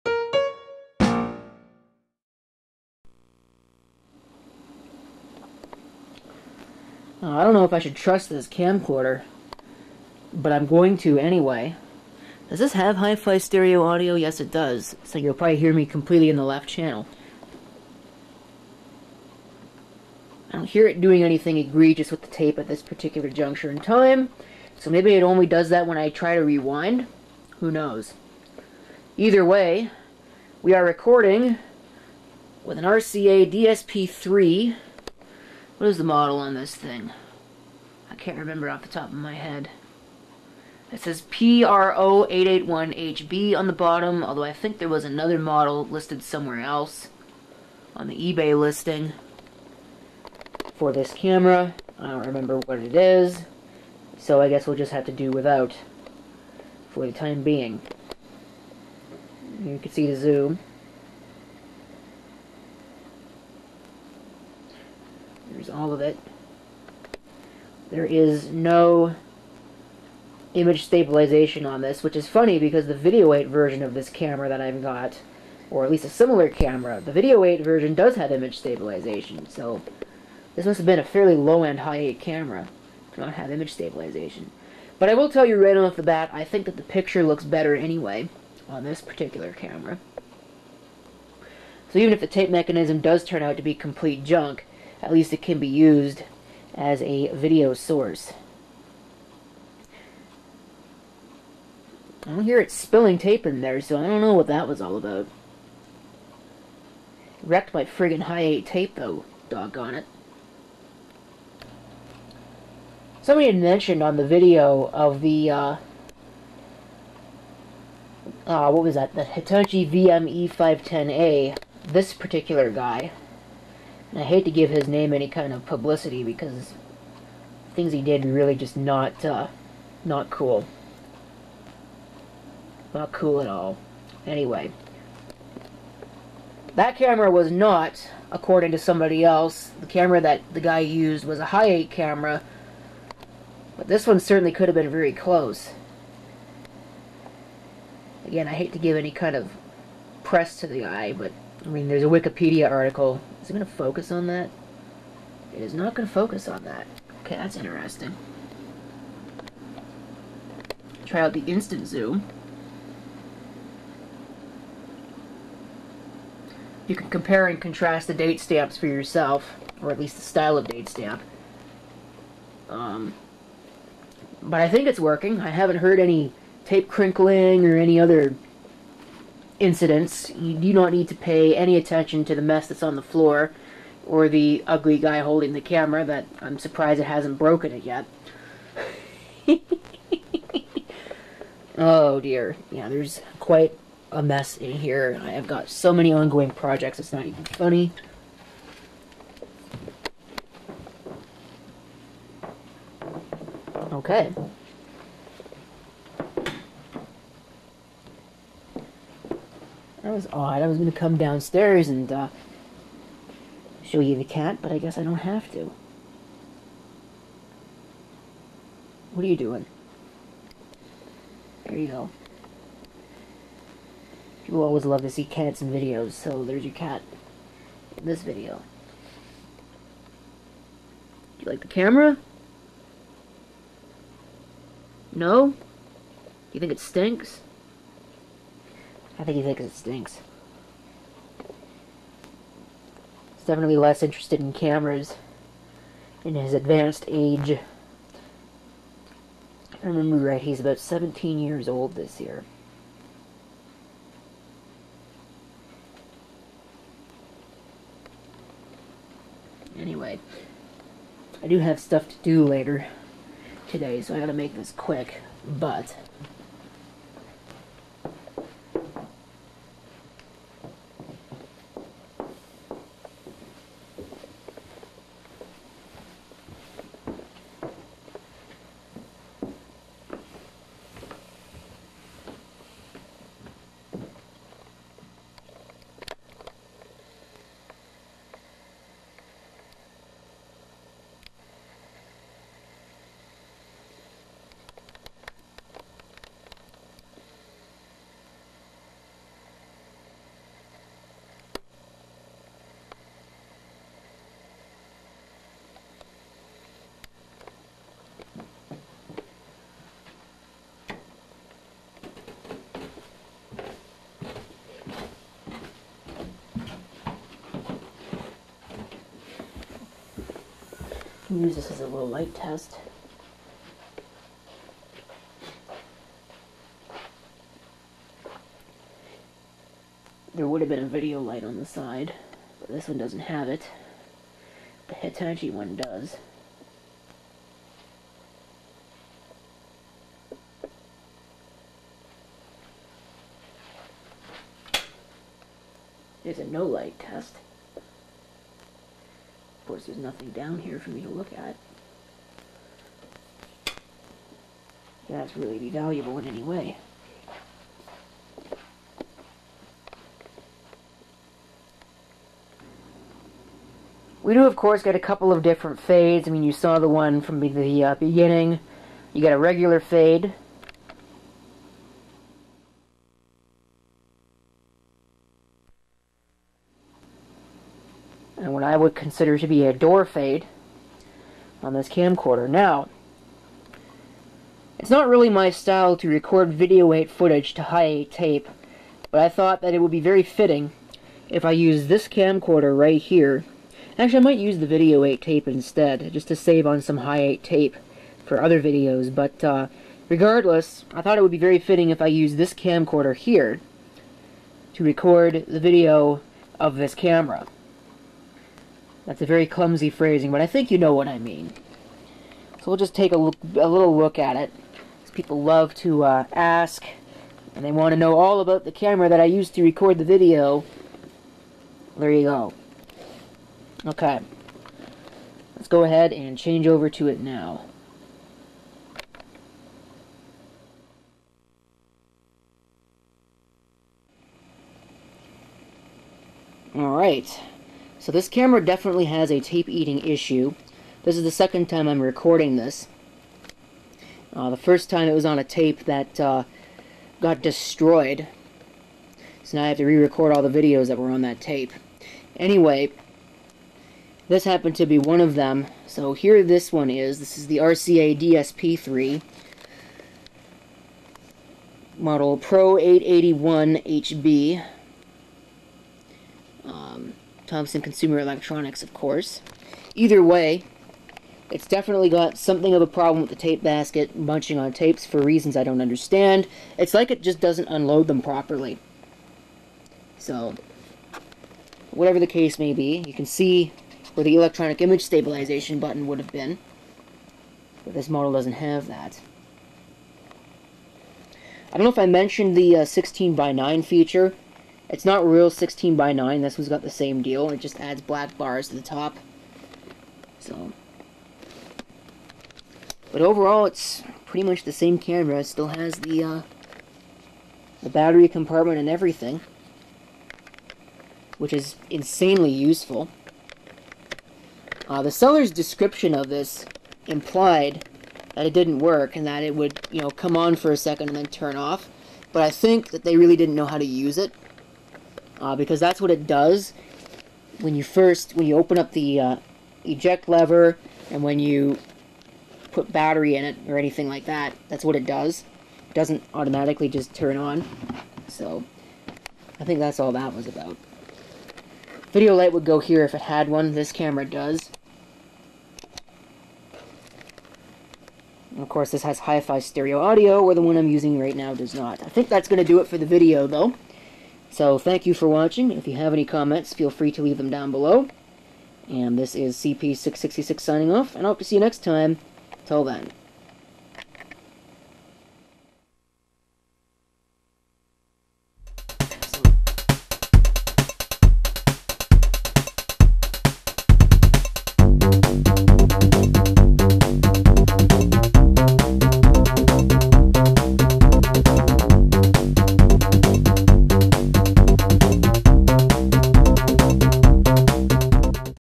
RCA DSP3 Pro881 Hi8 Camcorder
This one seems to have occasional issues unloading tapes, but otherwise works fine.